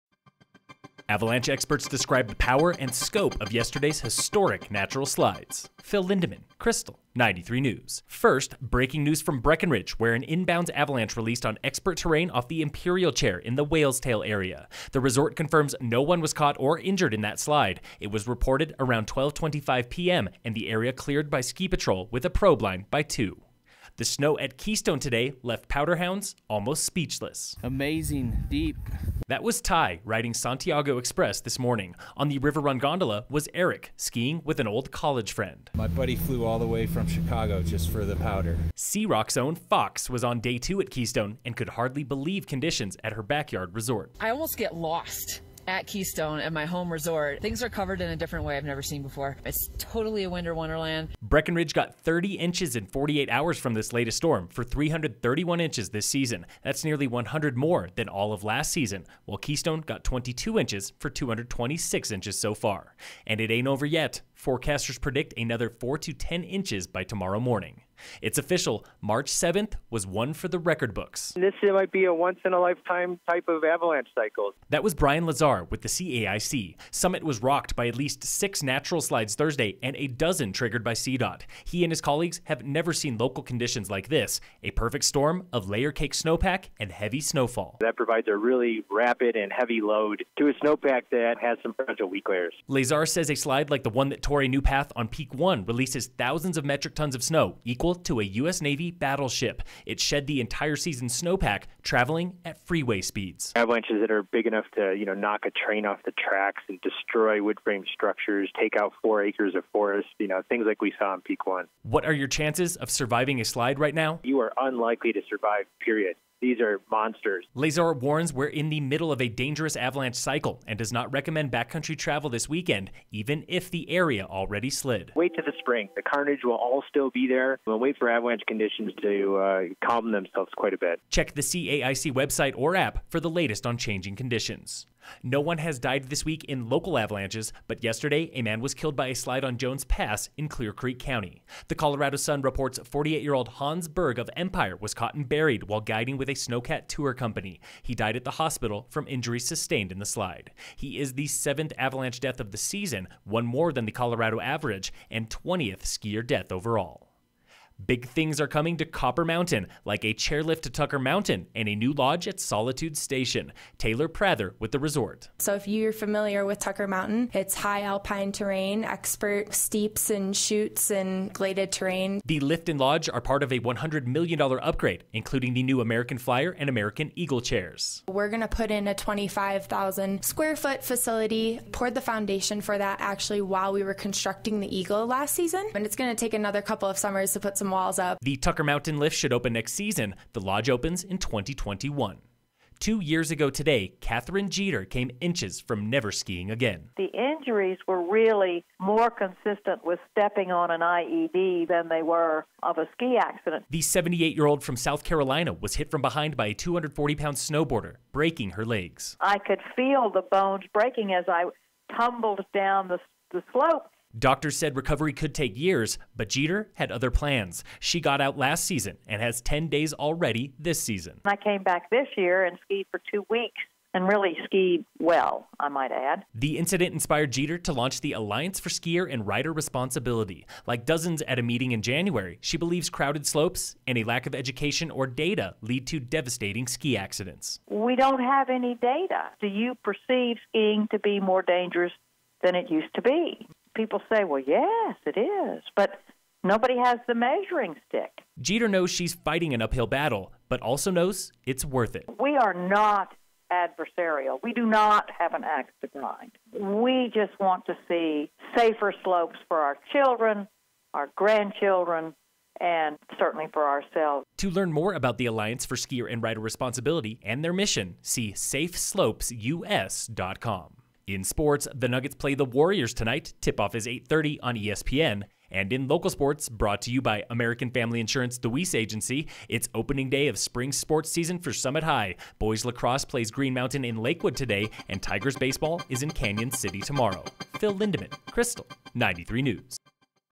'They are monsters': Experts describe the anatomy of historic CO avalanches... on Krystal 93 news
SEGMENTS

0:08 -- No on injured by in-bounds avalanche at Breckenridge
0:24 -- Pow day ecstasy at Keystone with 22 inches in 48 hours
1:08 -- CAIC forecasters explain the power and scope of 'historic' slides on March 7
2:15 -- Snowcat guide killed in avalanche on Jones Pass yesterday
2:37 -- Tucker Mountain chairlift, new Solitude Station lodge prat of $100M upgrades at Copper
3:12 -- How Katherine Jeter's traumatic ski injury lead to a new nonprofit for safety, responsibility
4:31 -- National sports
4:35 -- Local sports

Preview image by Summit Daily News.